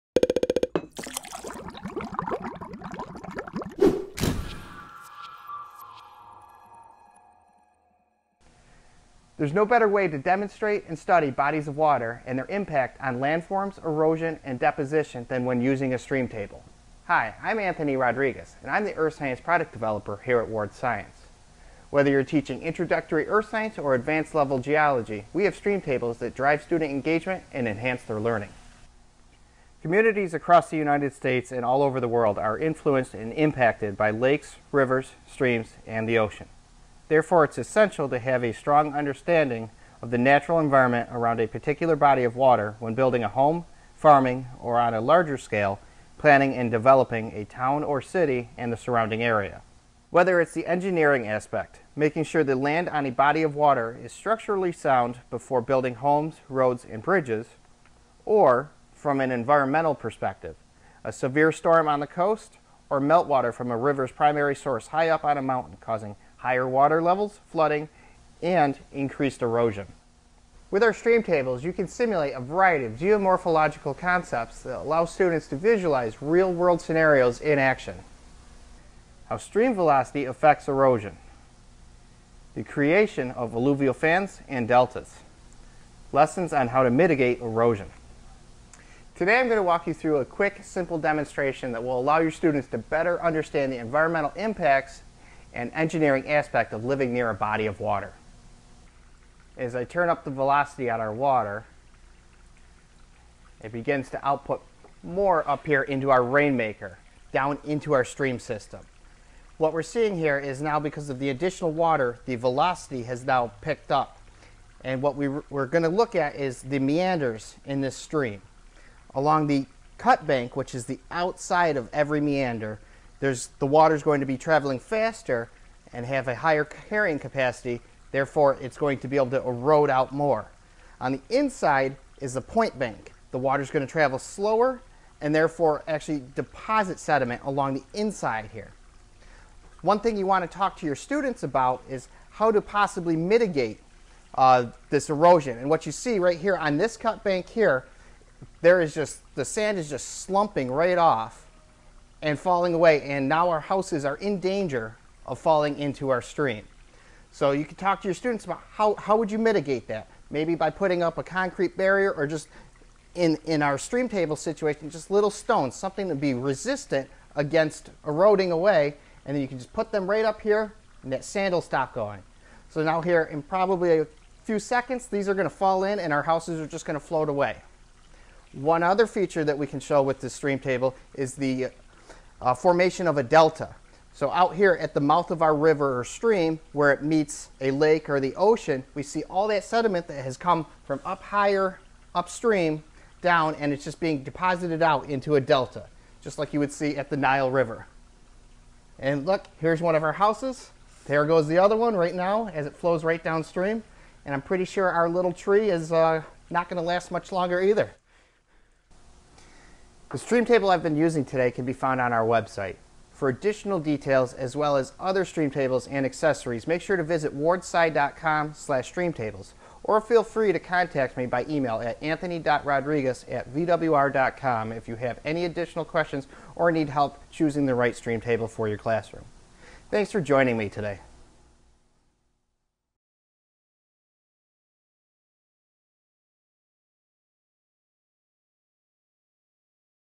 How to Use a Landform Simulator to Study Erosion, Deposition, and Bodies of Water
Teaching your earth and environmental science students about erosion, deposition, and the effects of bodies of water on land?

You could pack your bags and take a class trip to a river,  valley, or ocean - or, you could stay right where you are and use a landform simulator!

From rainfall and wave simulation to mountain peaks, volcanoes and meandering shorelines, landform simulators make it easy to model real-life topographic features and demonstrate the effects of weather, climate and water flow on the surrounding land and environment.

In this video, we'll show you how to use a landform simulator in your classroom to study a variety of earth and environmental science topics, and how to choose the best options and features for your classroom and budget.